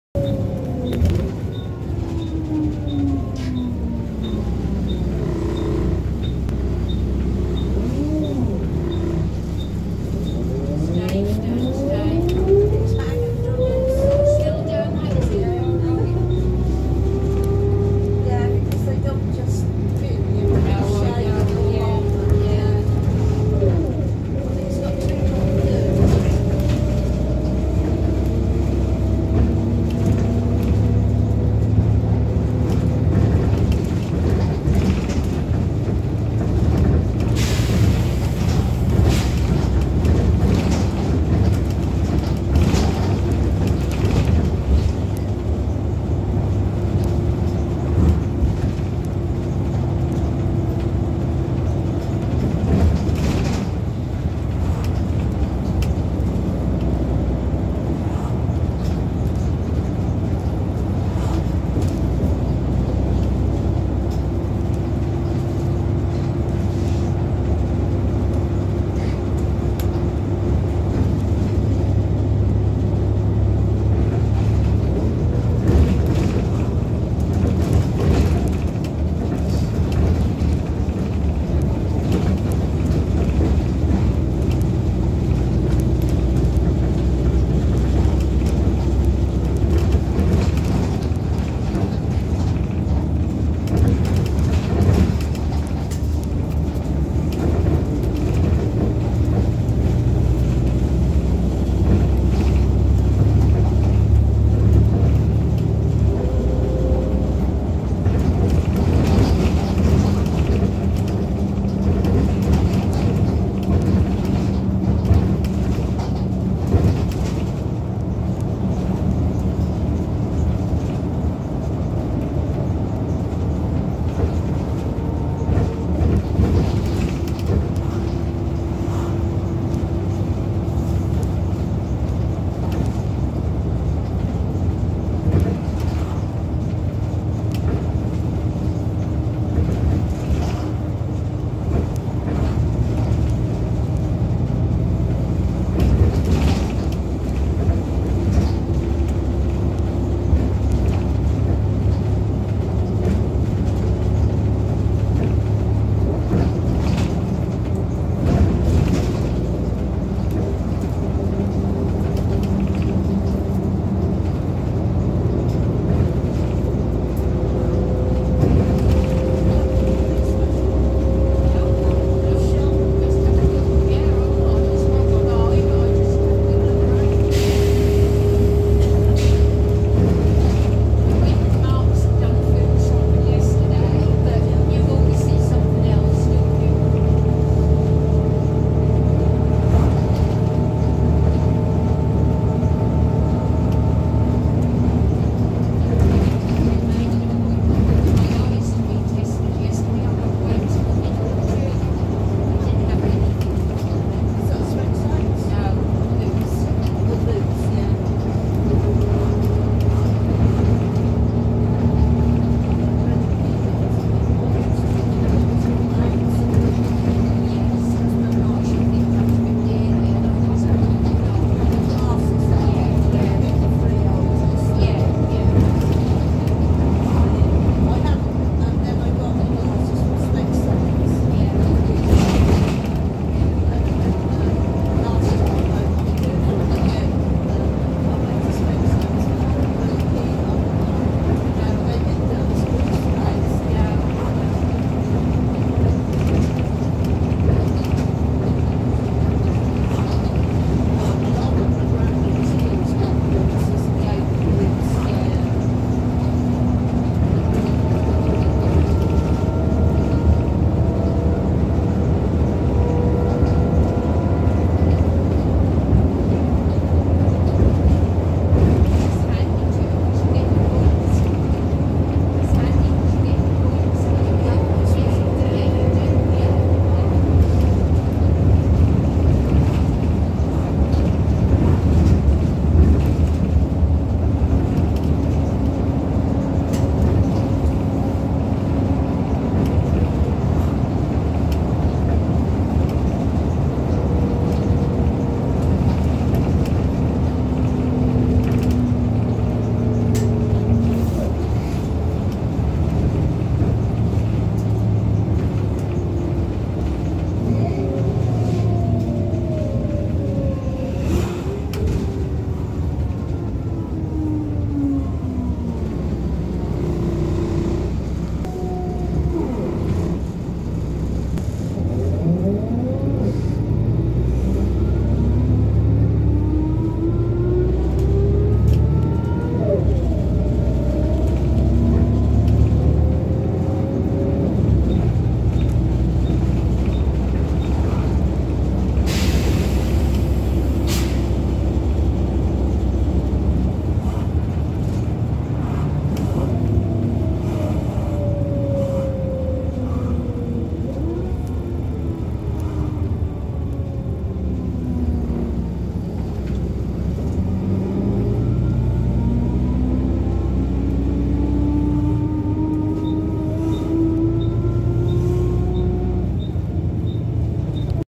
*Beautiful ZF!* Coach Services (Thetford) Volvo B7RLE Wright Eclipse (HF05 HXG)- Route 37A
Sorry this video is audio only. By the time the bus had got to me it had wormed its way through most villages in South Norfolk and was pretty crammed so there was nowhere suitable to actually film...

Absolutely fantastic sounding ex Morebus B7RLE. Cracking ZF and very strong, healthy sounding Volvo lump. Not massively fast despite running a bit late, but its fairly rare to have a B7RLE on here so just enjoy the marvellous allocation!

Operator- Coach Services Ltd
Bus- Volvo B7RLE Wright Eclipse
Reg- HF05HXG
Fleet no- N/A
Depot- Thetford
Ex- Go South Coast (Morebus)
Route- 37A (East Harling to Norwich via Mulbarton and other villages)